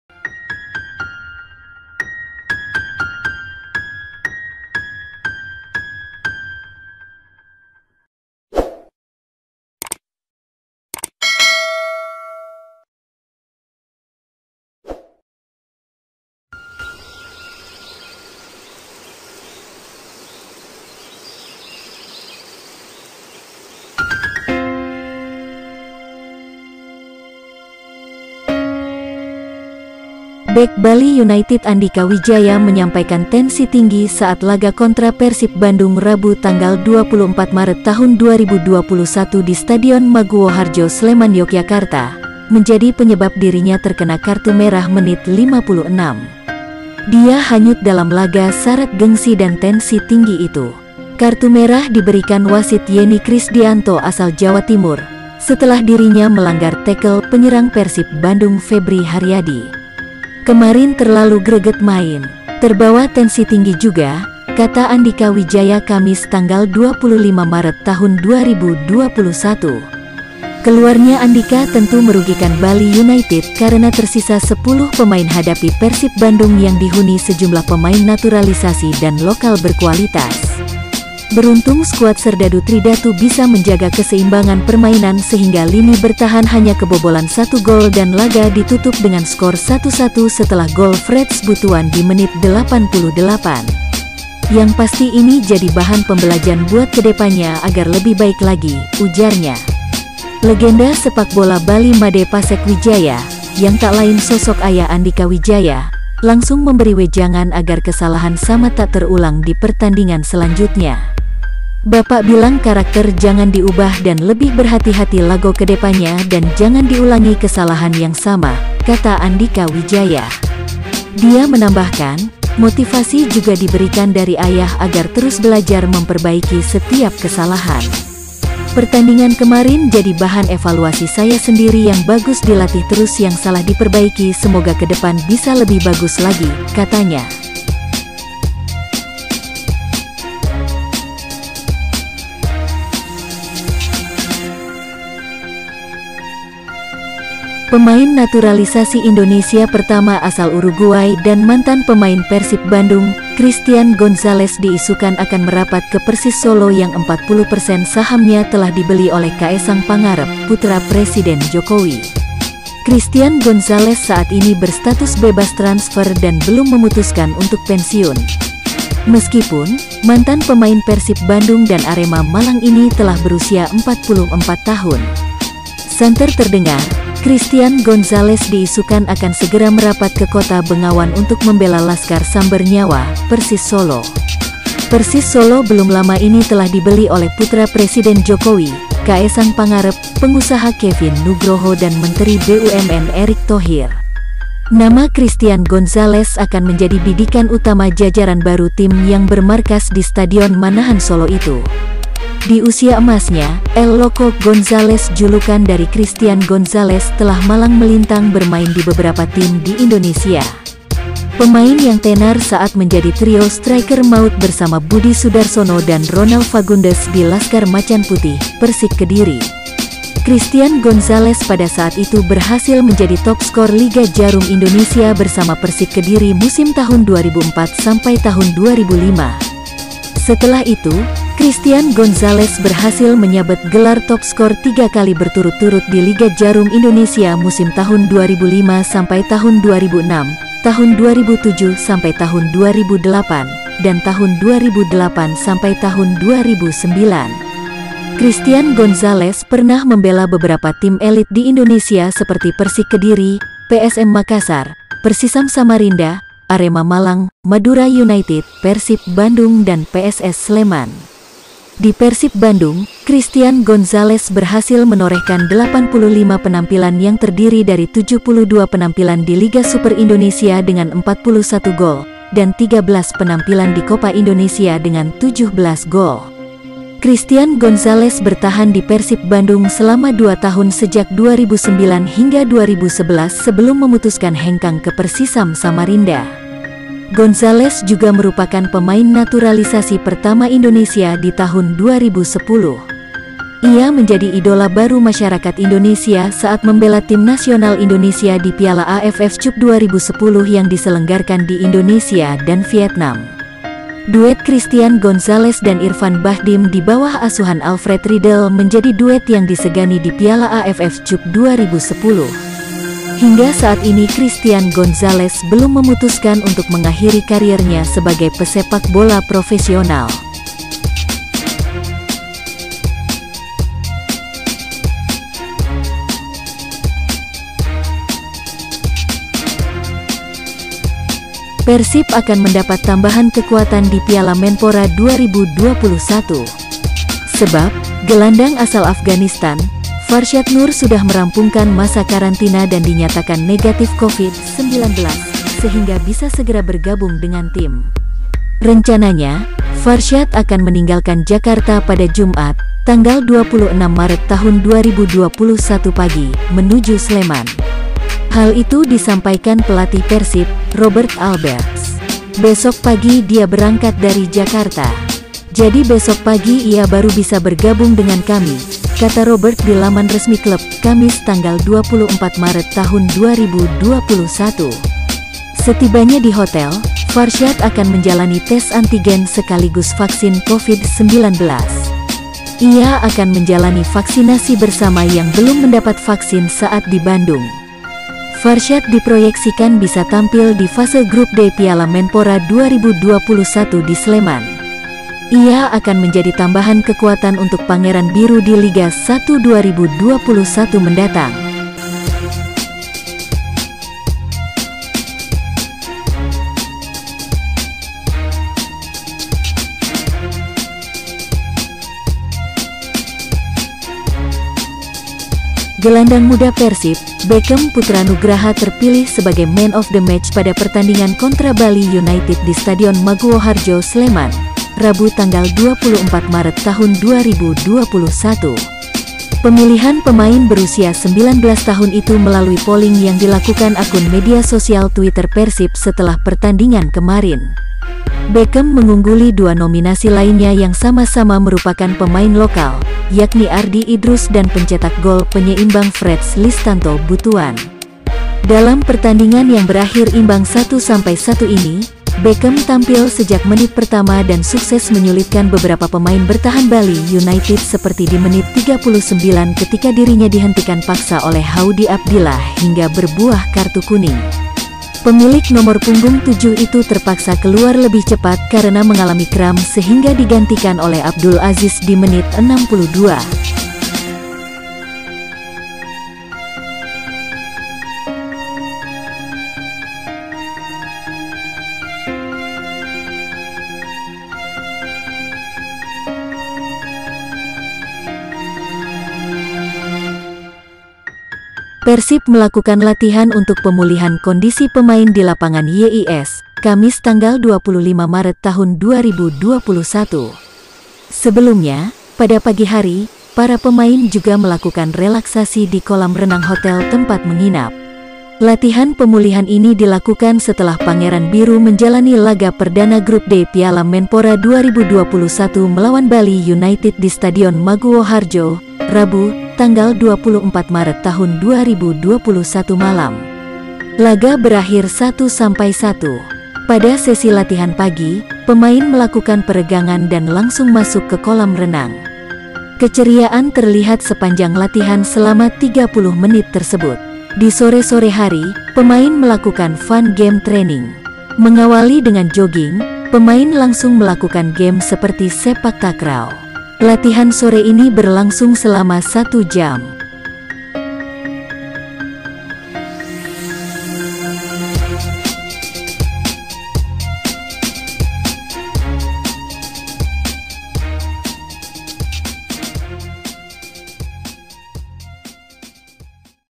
BERITA PERSIB TERBARU HARI INI 25 MARET 2021 PART 4
1. Andhika Wijaya Beberkan Alasan Tekel Penyerang Persib Febri, Terbawa Tensi Tinggi
2. Mantan Pemain Naturalisasi Persib Bandung Diisukan Merapat ke Persis Solo, Pernah Jadi Top Skor Liga
3. Persib Dapat Tambahan Kekuatan, Farshad Noor Besok Bergabung ke Sleman
4. Beckham Terpilih Jadi Man Of The Match Persib Kontra Bali united
5. Pulihkan Kondisi, Persib Gelar Sesi Latihan Ringan